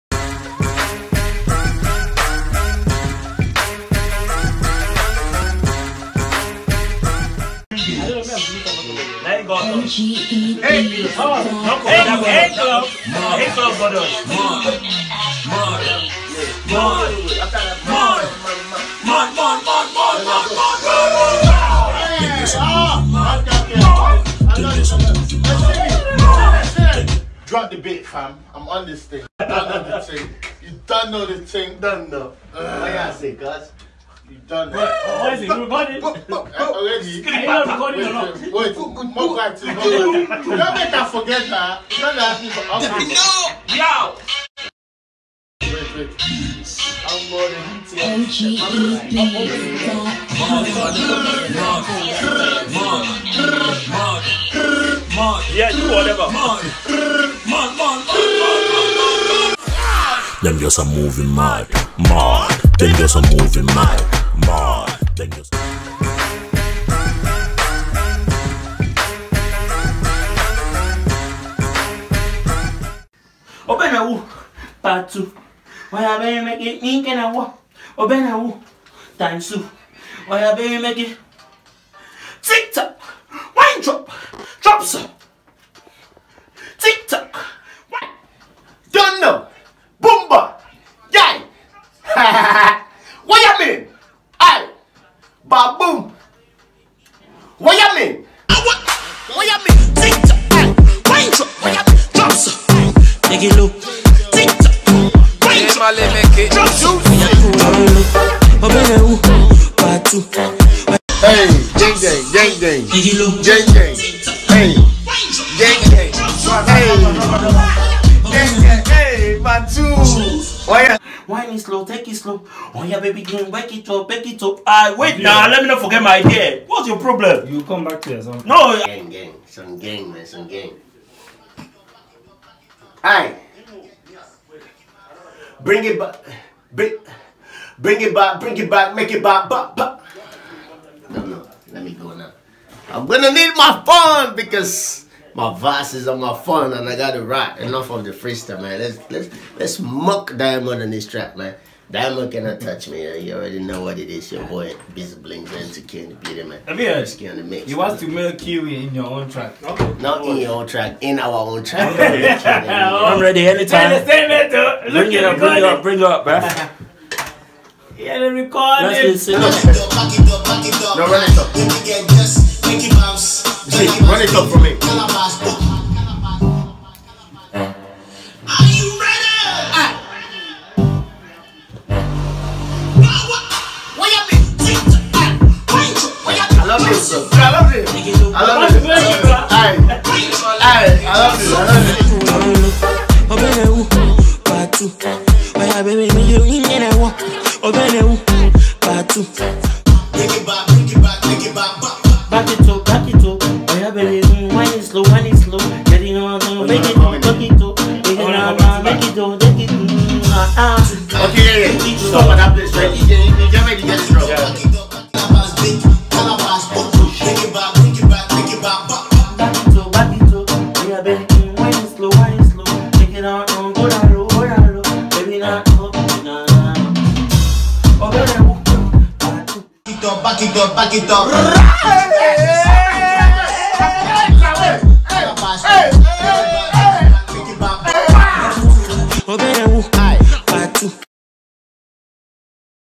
kicking it with N2K(Biza Blingza & Diamant B) in studio recording
Hey Guys, it's been a while but we are back with another episode of kicking it with N2K.
We are giving yews an exclusive insight into our new project, and hope you are entertained during this Period of Lockdown.
Please keep safe and God Bless